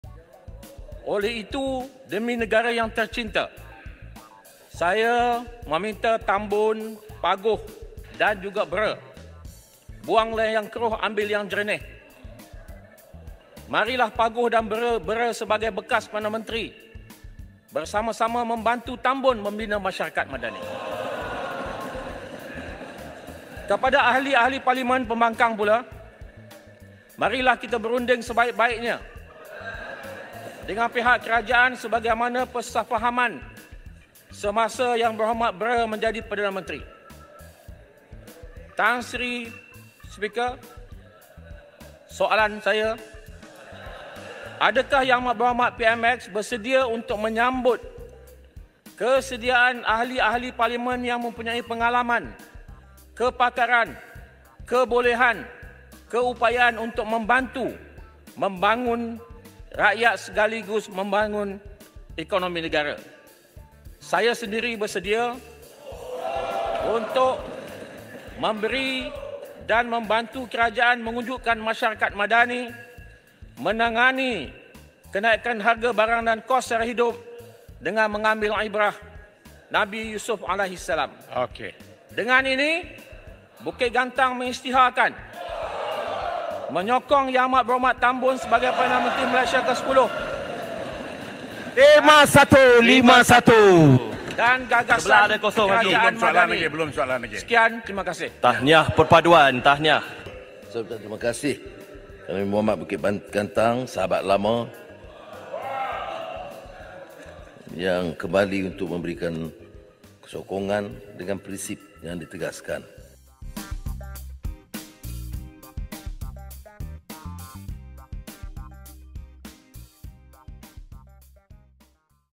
【国会一幕】土团第五任倒戈 安华道谢
土团党国会议员倒戈风持续发酵，继4名土团党议员陆续宣布支持安华领导的团结政府后，今日又有一名土团党YB倒戈挺安华，而他就是武吉干当区国会议员赛阿布胡申。